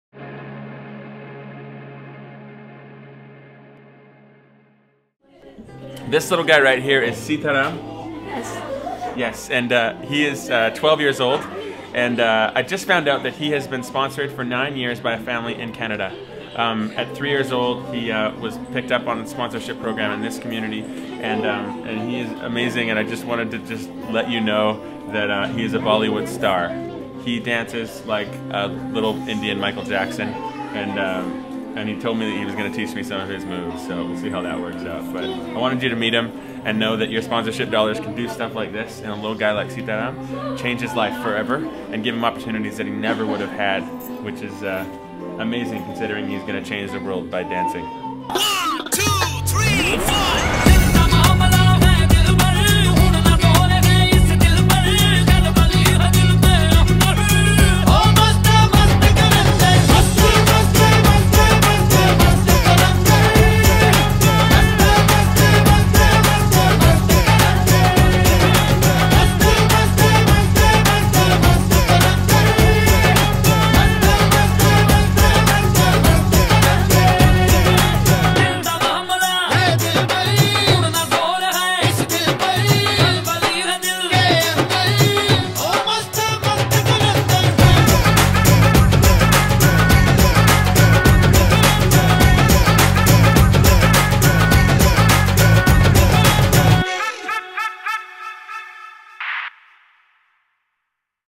Dancing with the Starfield | World Vision
Starfield get a dancing lesson from 12-year-old Sitaram, a future Bollywood star and sponsored child, during a visit to India with World Vision Canada.

to make a meaningful change today.